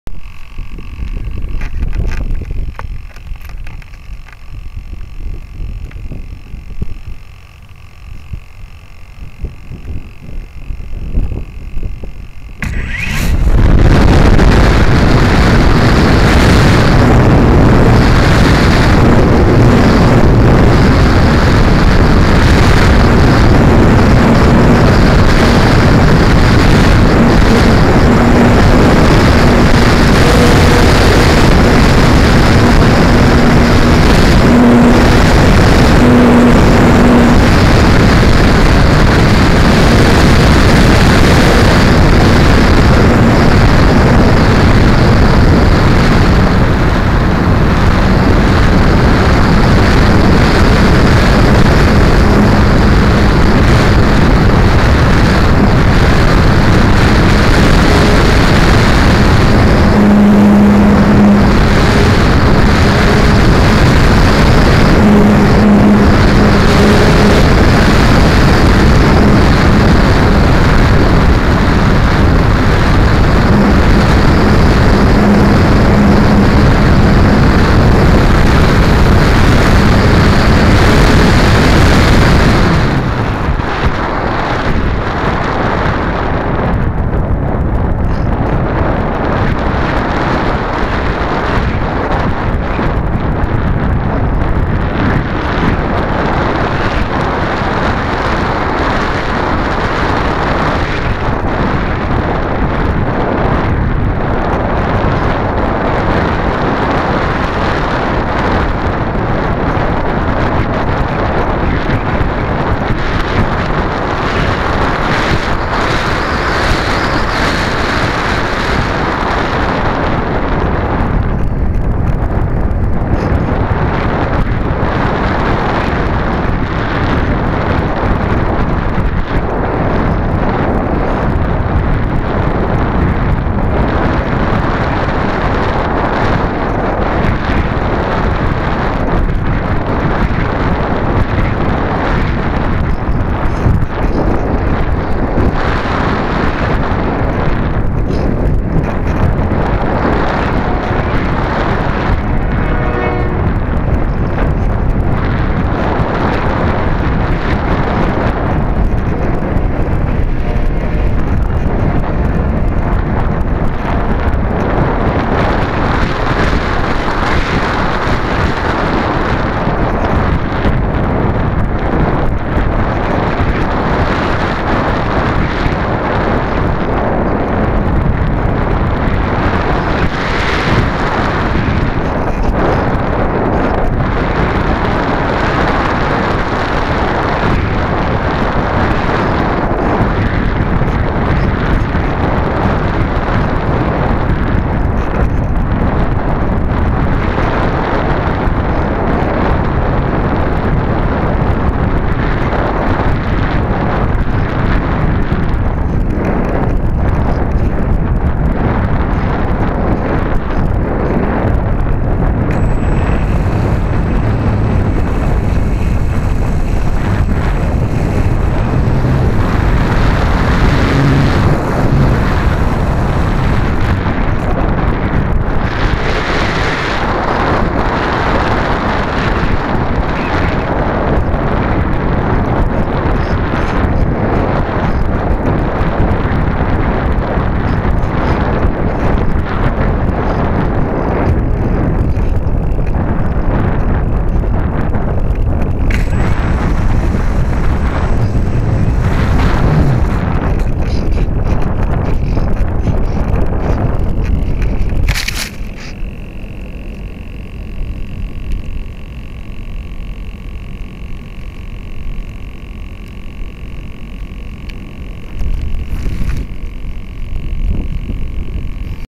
Radian over Cabot, AR on a windy November day in 720p
This was the first test of a camera with 720p HD mounted on the back of a PZ Radian sailplane.  The flight took place near a construction site on the backside of Cabot Junior High North in Cabot, AR.  The flight was shortened and a safe landing was made earlier than expected due to high winds encountered at altitude.